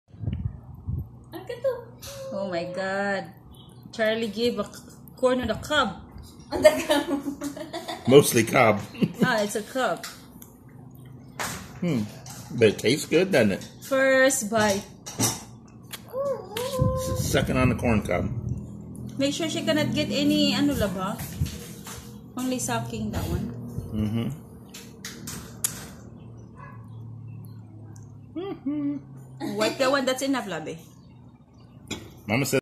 BABY EATING CORN ON THE COB
We let her try if she like and it seems she did like it..